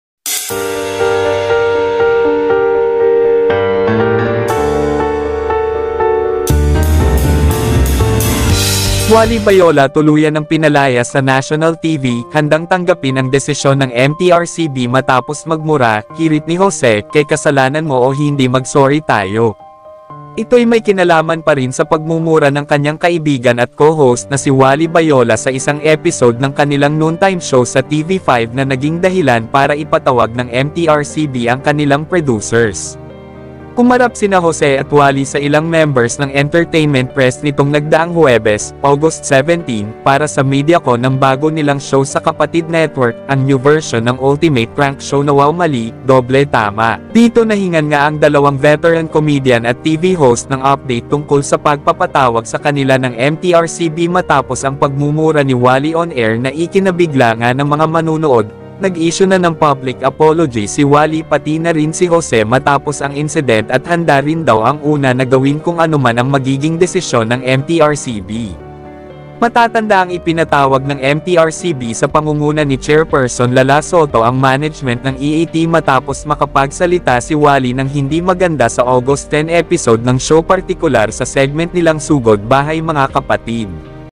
TULUYAN ng PINALAYAS ‼️Wally Bayola ng MTRCB ‼️Jose PUMALAG sa DESISYON!!OMG!!
Wally Bayola TULUYAN ng PINATANGGAL ng MTRCB!OMG!!